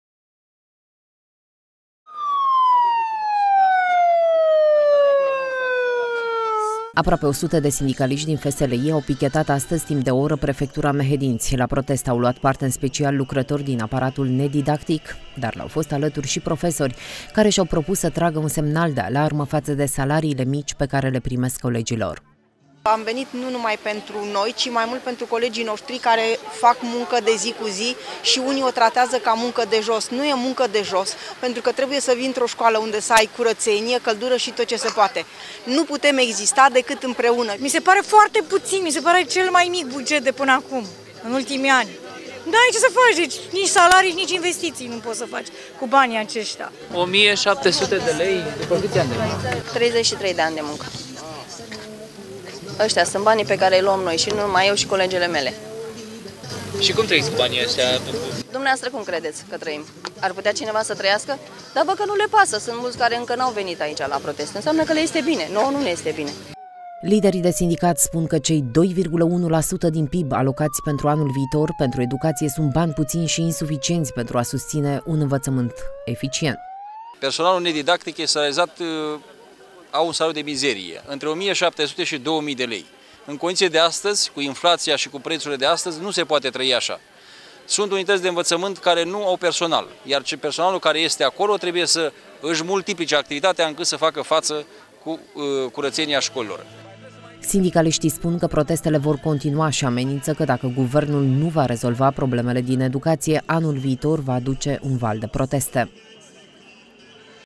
PERSONALUL NEDIDACTIC, ÎN STRADĂ
Sindicaliștii din învățământ au ieșit în stradă și la Drobeta-Turnu-Severin, în semn de protest față de salariile mici pe care le are personalul nedidactic din sistem. Oamenii au reclamat faptul că după zeci de ani în care activează în învățământ, au venituri de doar 1.700 de lei, iar în condițiile scumpirilor din ultima perioadă, este tot mai dificil să-și asigure traiul de zi cu zi.

Aproape 100 de sindicaliști din FSLI au pichetat în cursul acestei zile, timp de o oră Prefectura Mehedinți. La protest au luat parte în special lucrători din aparatul nedidactic, dar le-au fost alături și profesori, care și-au propus să tragă un semnal de alarmă față de salariile mici pe care le primesc colegii lor.

Liderii de sindicat spun că cei 2,1 % din PIB alocați pentru anul viitor, pentru Educație sunt bani puțini și insuficienți pentru a susține un învățământ eficient.

Sindicaliștii spun că protestele vor continua și amenință că dacă Guvernul nu va rezolva problemele din educație, anul viitor va aduce un val de proteste.